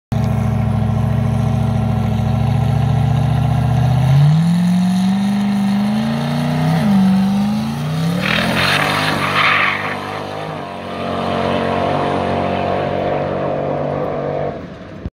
first test pass with the 5.3 turbo model A
first pass on a track with the lm7 turbo model A, only on 11psi still for test passes
tuned for 18, on e85 stock 200k bottom end truck motor with ss2 cam, sep1500 injectors on holley termi